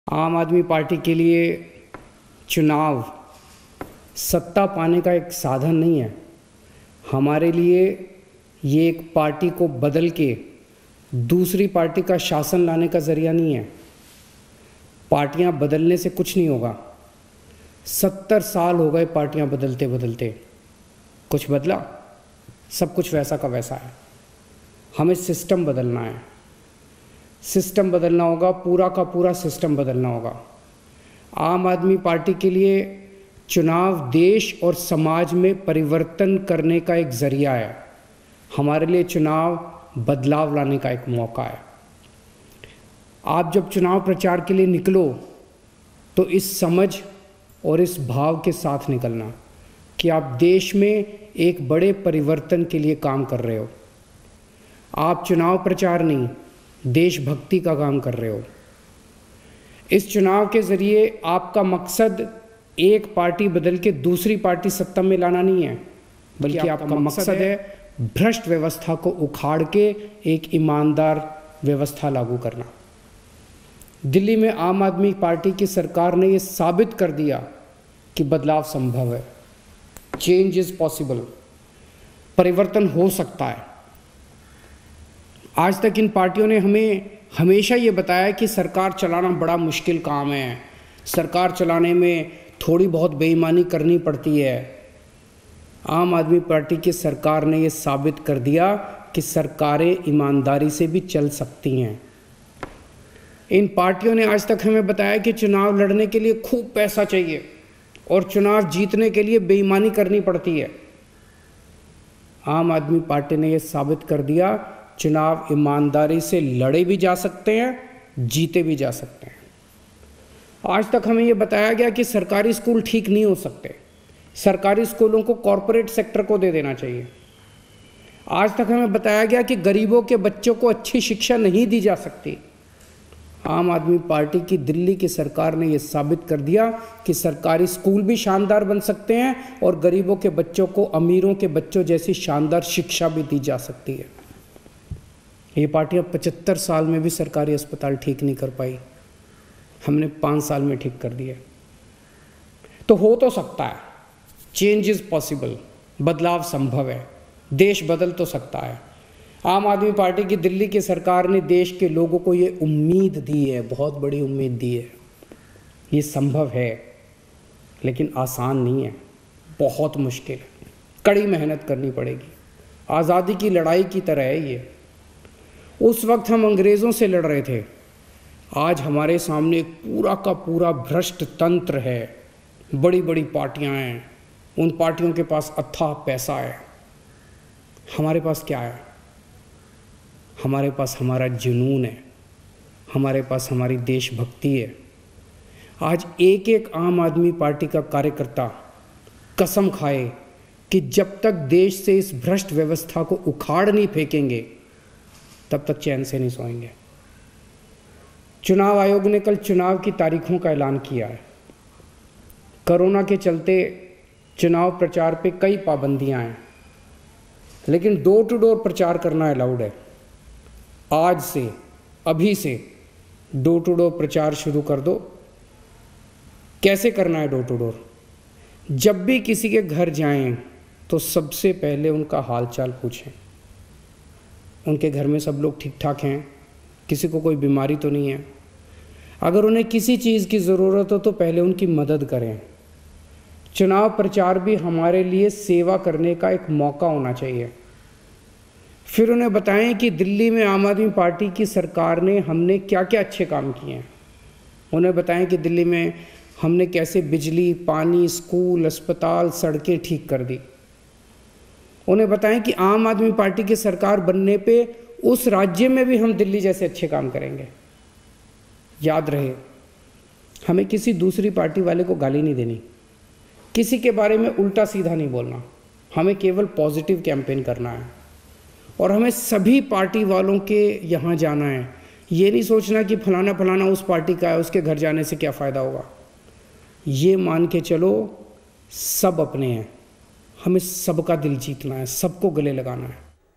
आम आदमी पार्टी के सभी कार्यकर्ताओं के नाम CM Kejriwal का संबोधन
News State Apps डाउनलोड करें:
Download Android and iOS Apps for more updates: